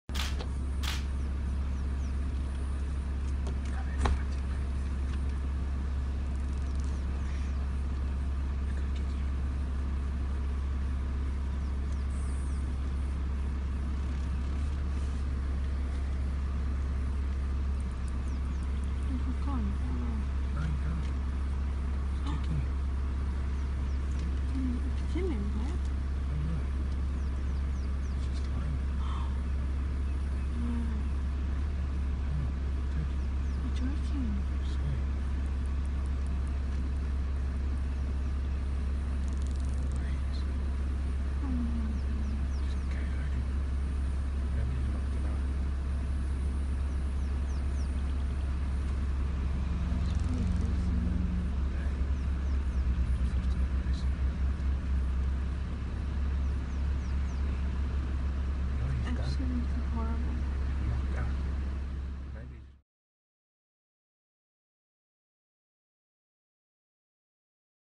Giraffe Knocks Out Rival and is Confused
When Giraffes fight, it always looks like a graceful dance in slow motion, but when one of the horns connects with a clean hit, the damage can be brutal. These 2 Giraffes were fighting when just that happened. A clean horn shot to the back of the head knocked the one Giraffe out cold, leaving the victor looking quite confused as to what had happened.
A few minutes later, the knocked out Giraffe got back up and walked it off, accepting his loss.
This was a special sighting that very few people get to see, so well done to Darren Thomas, and thanks for the amazing footage!